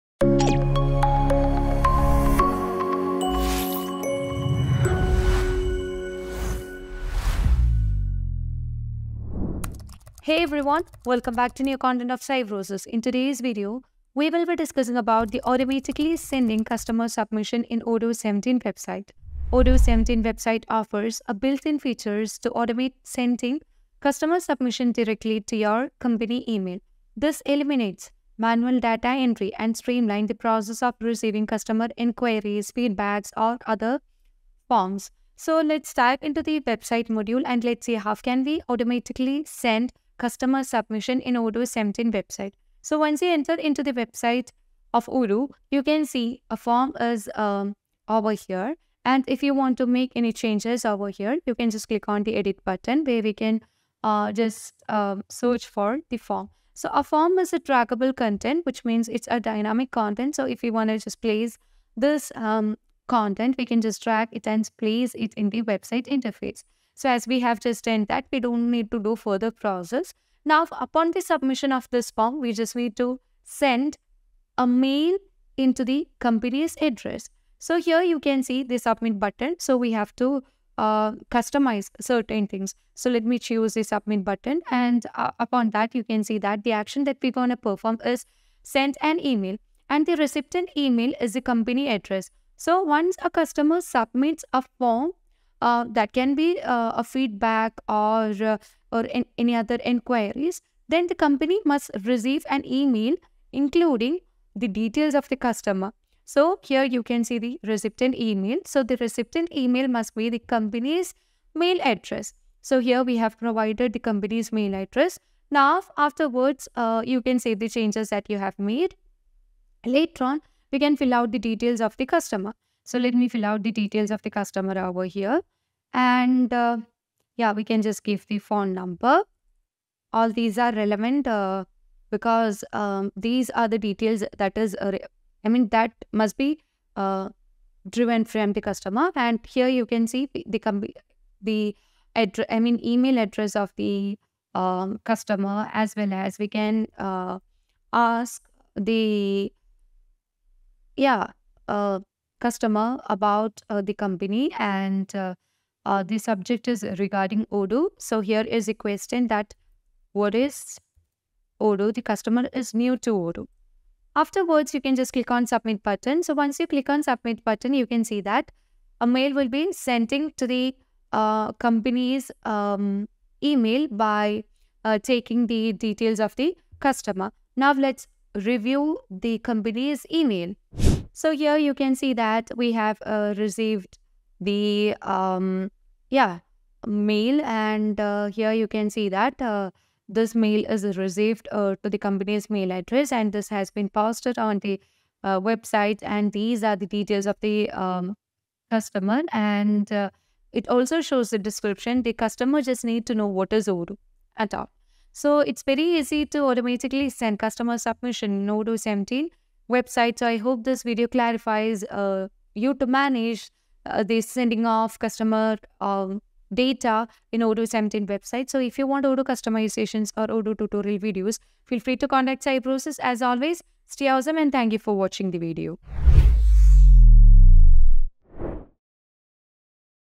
How to Automatically Send Customer Submissions in Odoo 17 Website App | Odoo 17 Functional Tutorials
Odoo 17 website offers a built-in feature to automate sending customer submissions directly to your company email. This eliminates manual data entry and streamlines the process of receiving customer inquiries, feedback, or other forms.

There are two main reasons why you might receive emails from an Odoo website form:

Contact Form Submission: Odoo is a popular open-source business application suite that includes a website builder. Many Odoo websites include contact forms that allow visitors to send messages or inquiries. When a visitor submits a contact form, Odoo is configured to automatically send an email notification. This email typically goes to the website owner or a designated recipient, and it includes the information submitted by the visitor on the form.

System Notifications: Odoo can also be used to send automated system notifications. For instance, if you create a new user account on an Odoo website, you might receive a welcome email generated by the Odoo system. These notifications are separate from contact form submissions and can be related to various Odoo functionalities.

In essence, Odoo emails stem from either a website visitor interacting with a contact form or the Odoo system itself sending automated notifications.